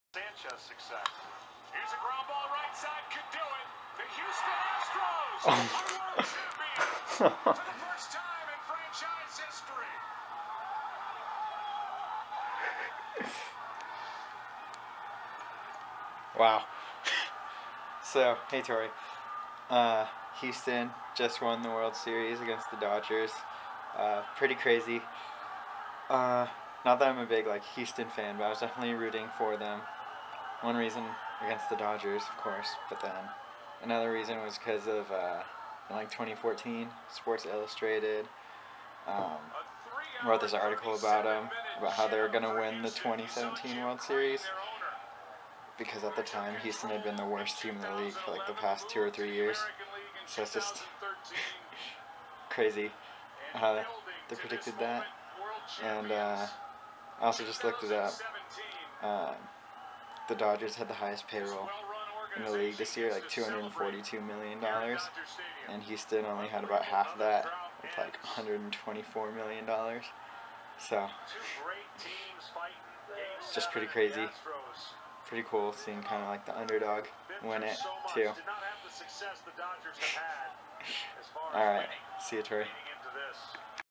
Dodgers Astros World Series Reaction
The Houston Astros just won game 7 of the World Series against the LA Dodgers!  Here's my reaction to the Astros winning their first ever title, and while they aren't really my team, I will root against the Dodgers,  It's also cool because in 2014 Sports Illustrated published an article saying that they would win the 2017 world series despite being the worst team in the league the year prior.  Crazy